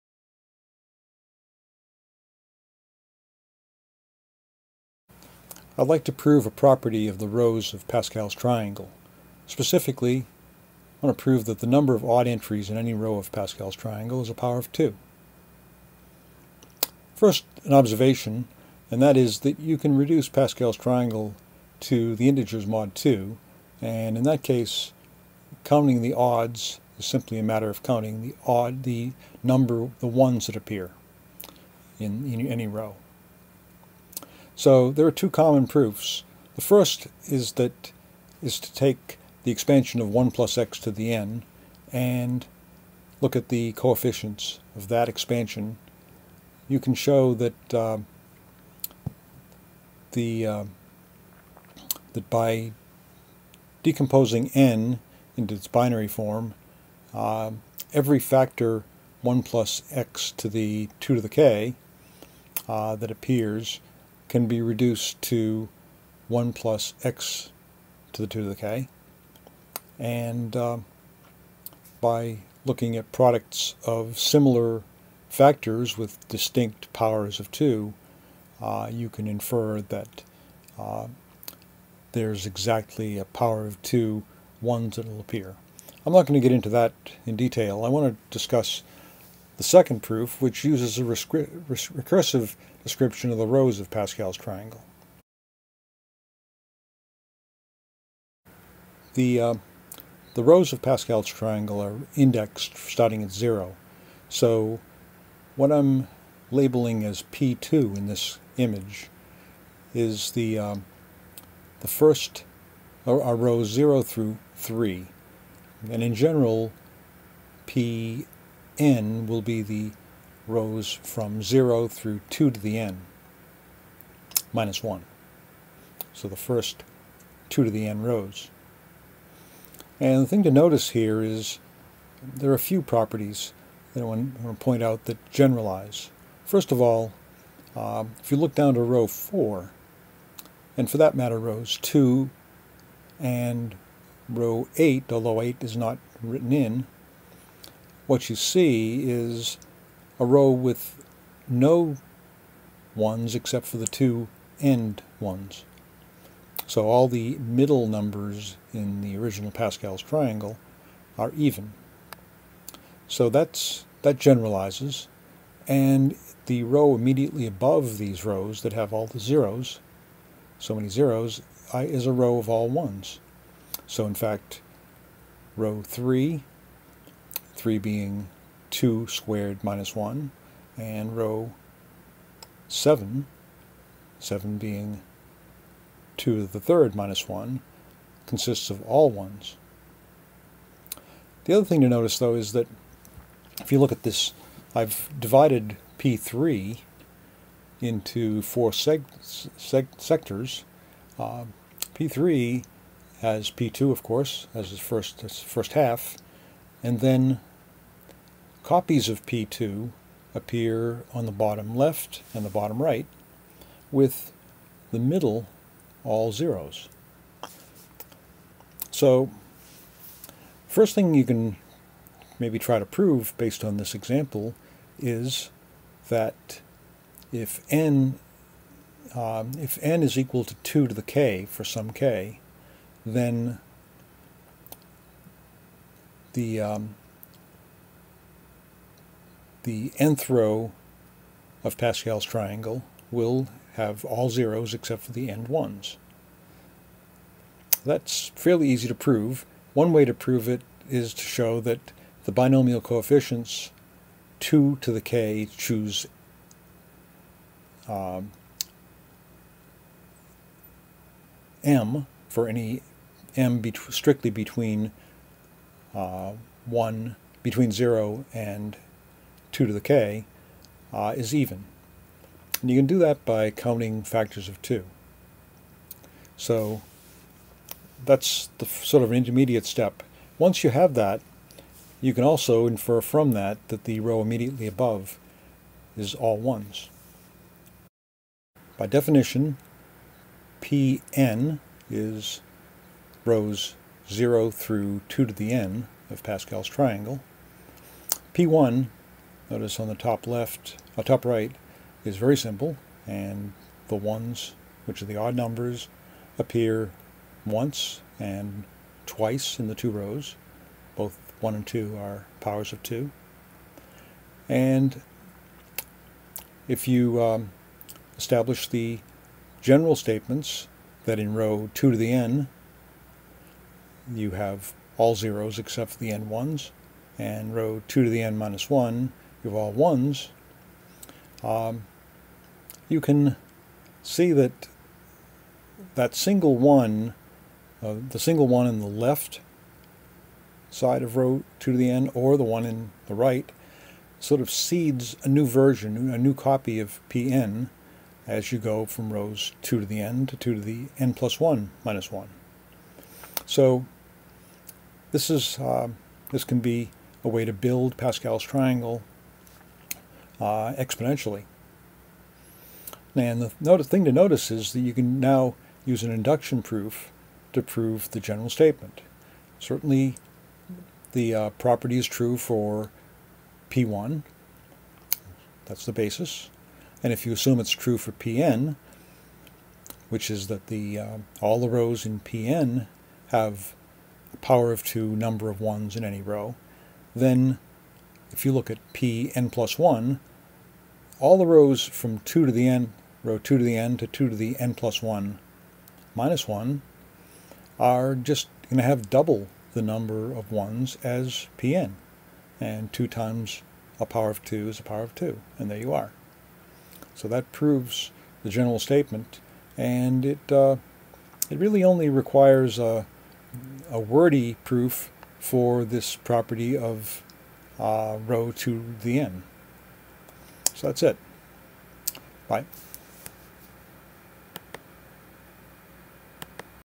Odd numbers in rows of Pascal's Triangle
This video outlines a proof that in any row of Pascal's Triangle, the number of odd numbers is a power of 2.